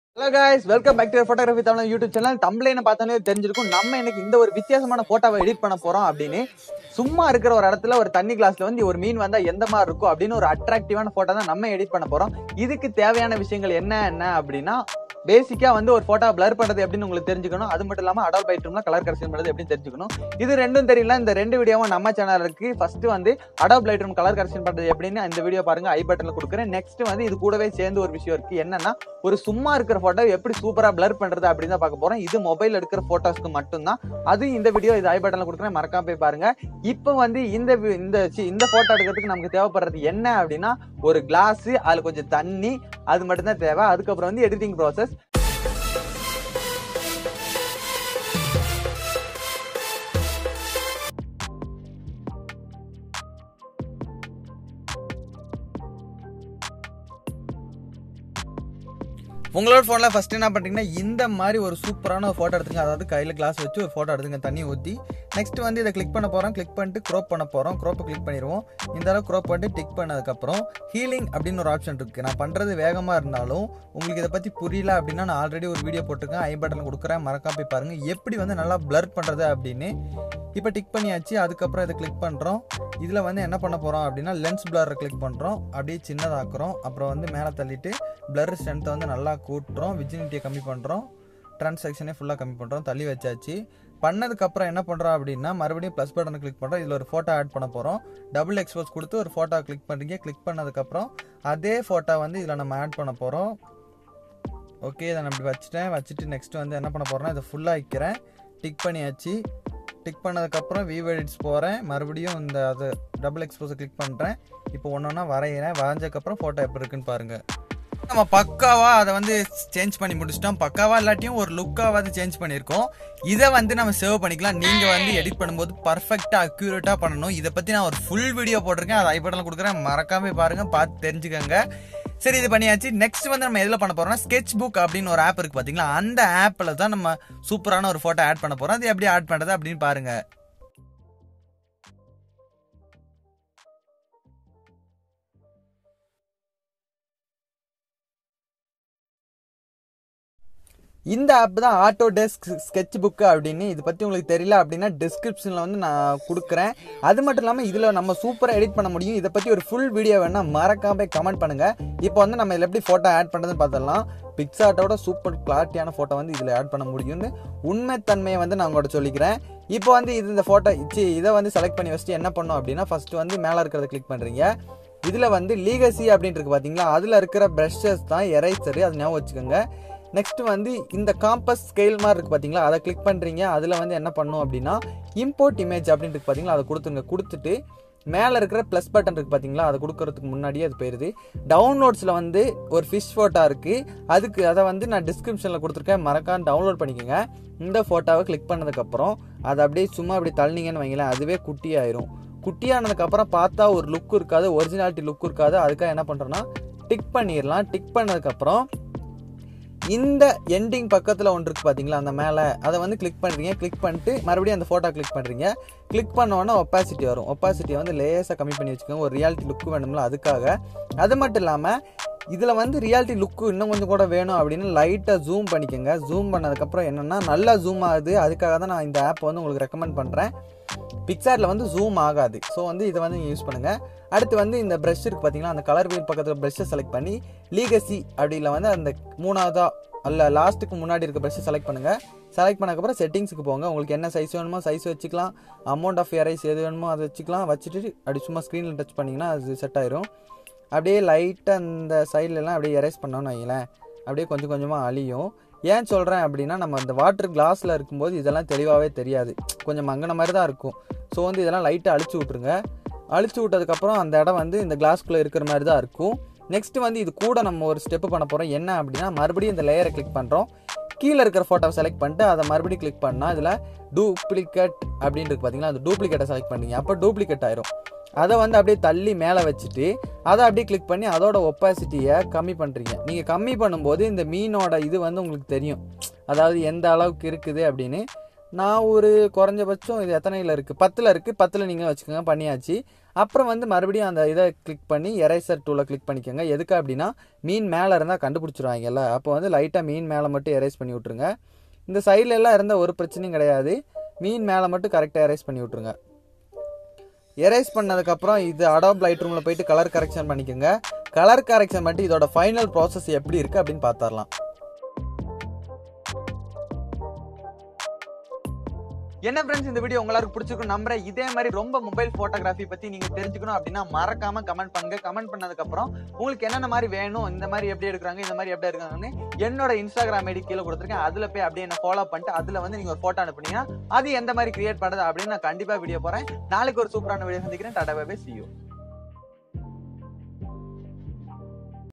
Fish photo editing mobile | snapseed | mobile photography Tamil | photography tamizha | mp_wala - 2
this video explain about how to creat fish photo editng in mobile its an mobile photography channel and tamil photo editing channel new mobile photography ideas in tamil subscrim\be my channel in ths video iam usinga two apps hance snapseed and sketch book